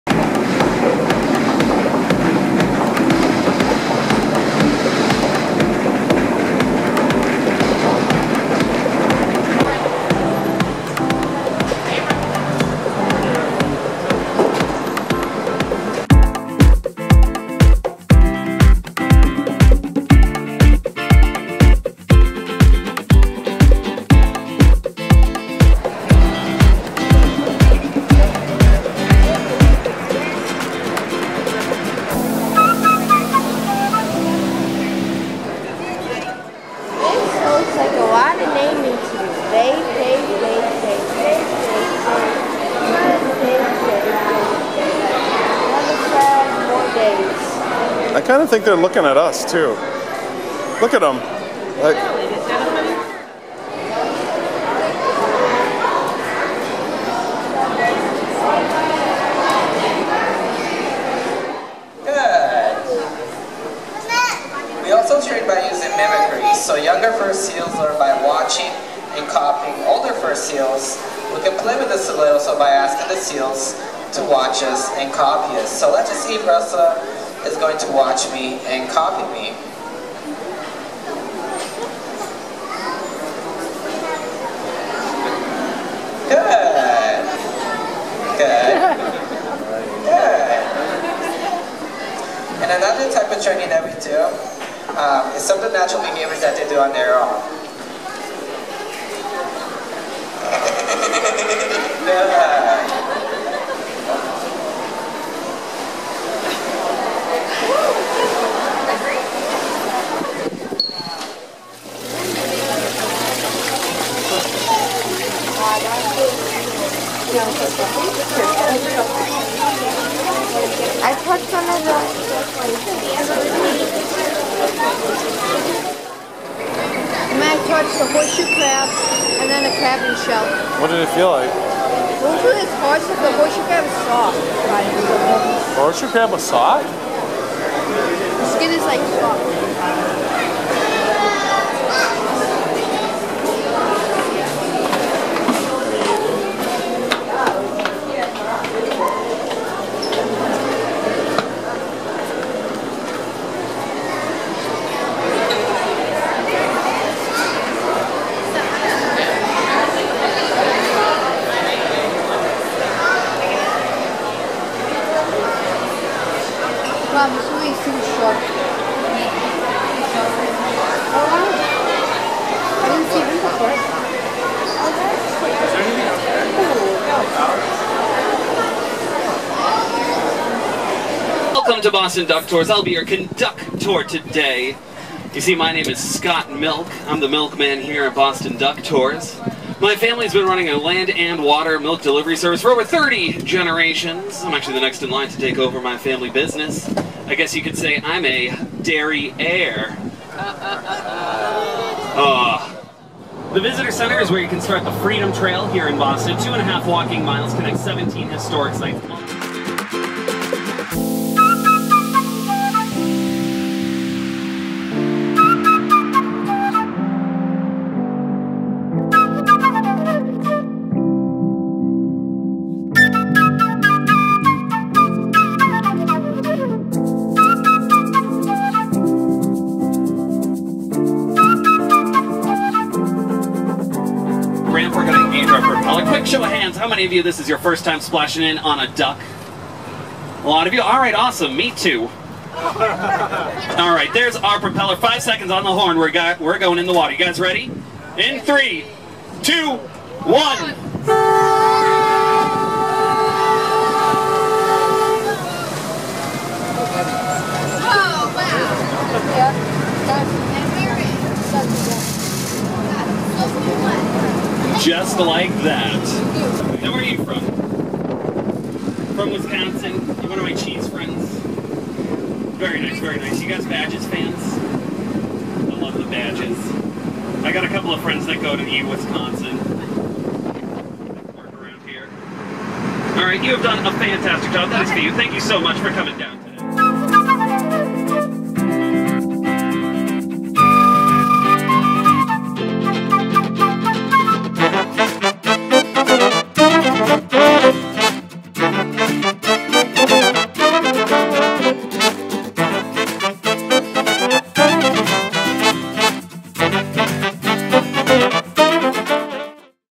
She DROVE a Duck!
Today we spent Father's Day in Boston checking out the harbor and beyond.